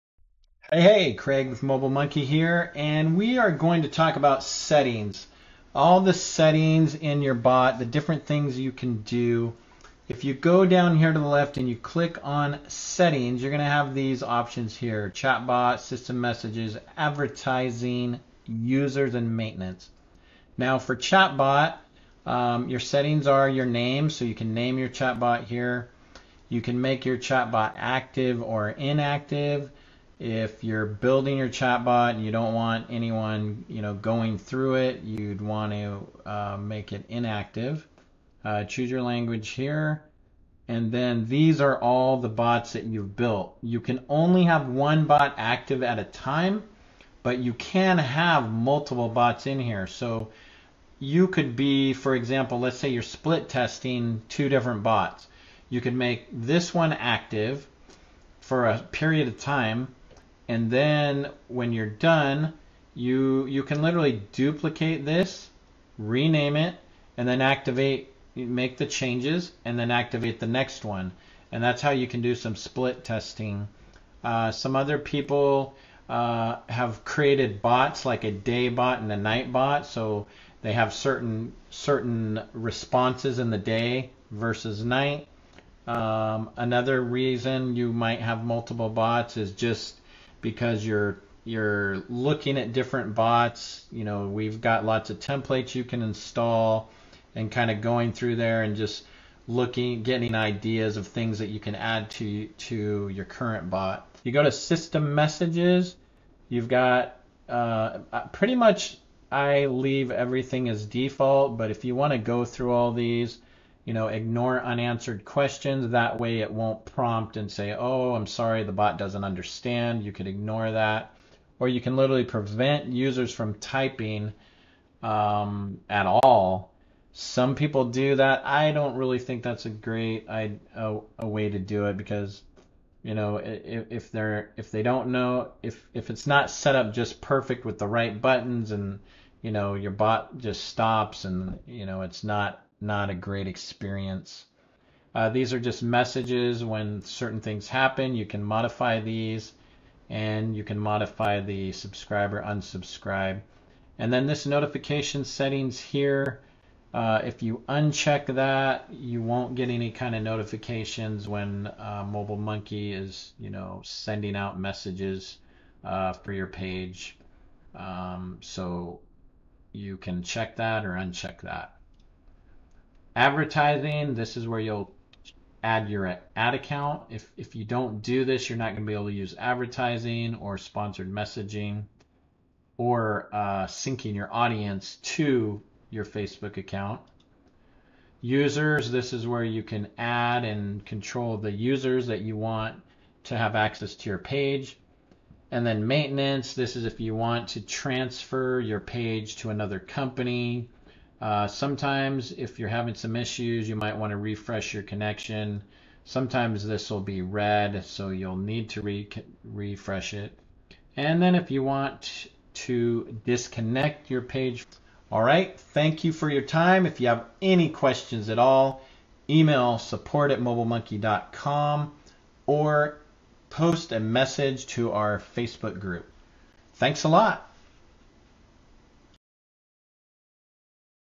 Chatbot Settings
An overview of the chatbot settings in your dashboard.